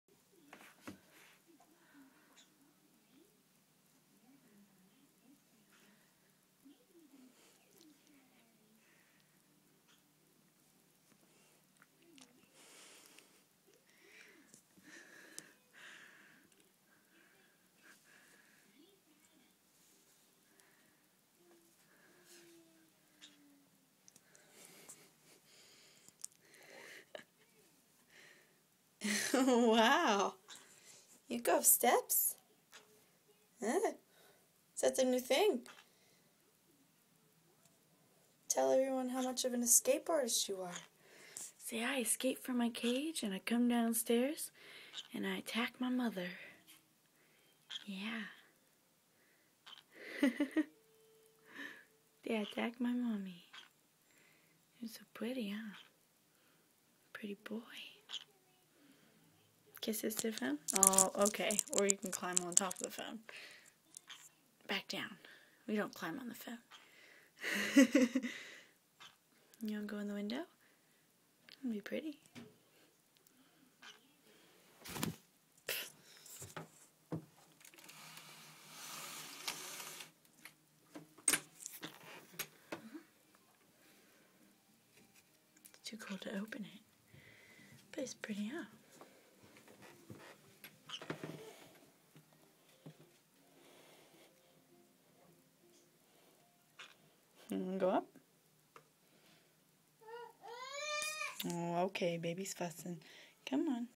Conure stair clime
Neeko following me upstairs lol